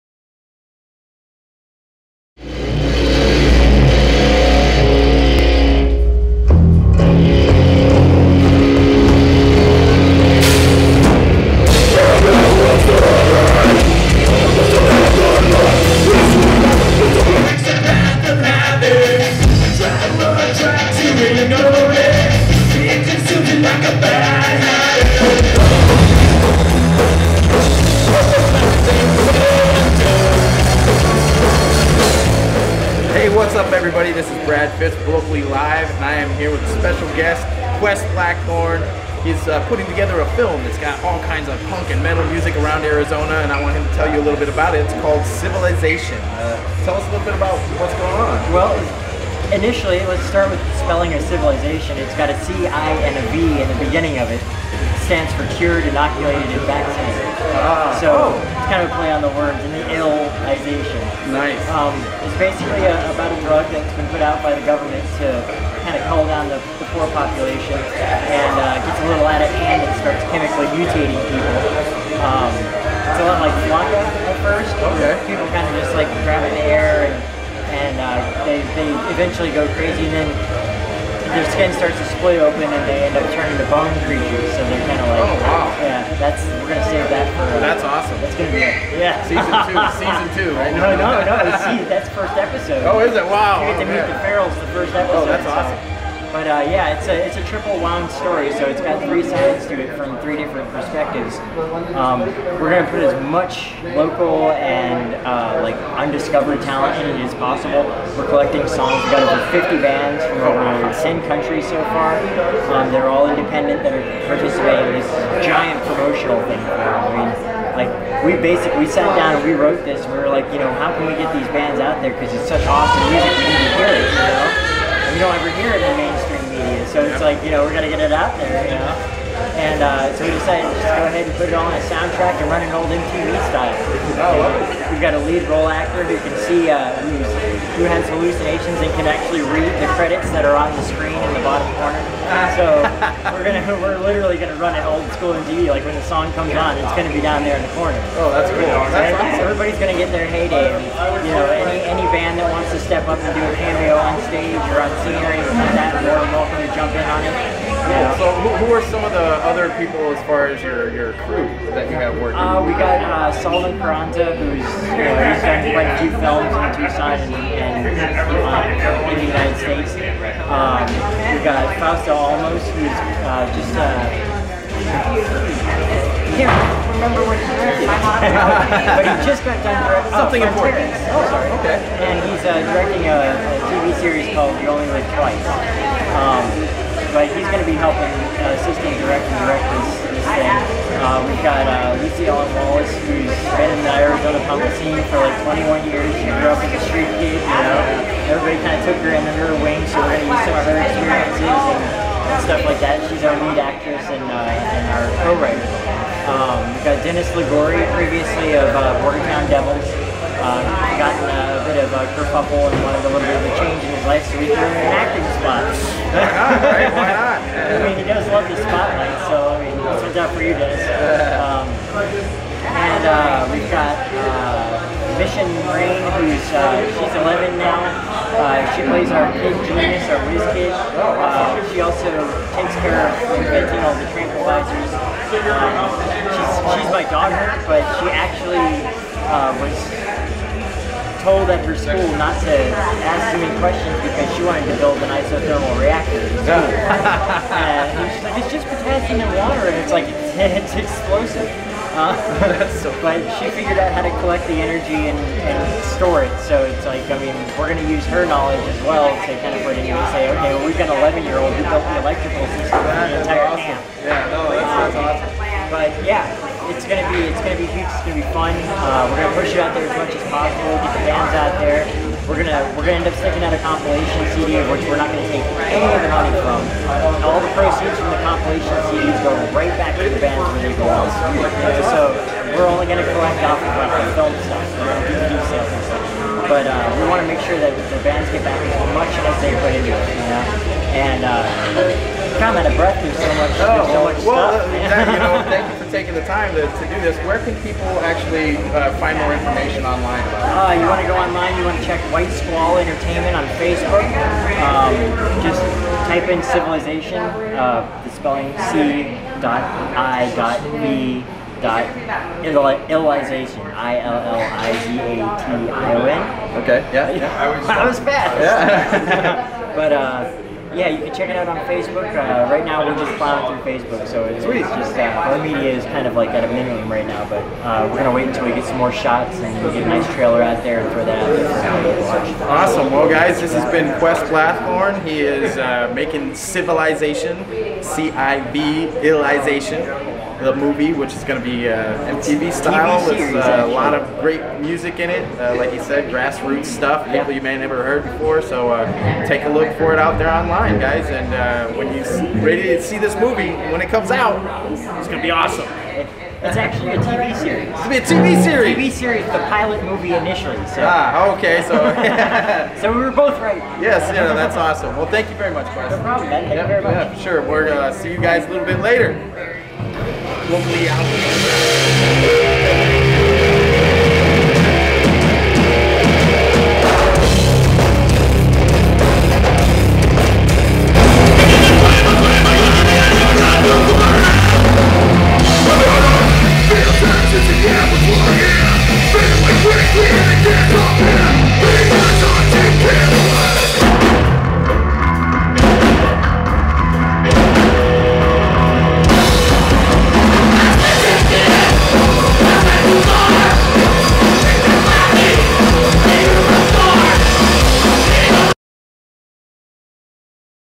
CIVillization Fundraiser at House of Bards, Interview with Quest Blackthorn
CIVillization Fundraiser at House of Bards, Interview with Quest Blackthorn, Creator of the CIVillization movie project.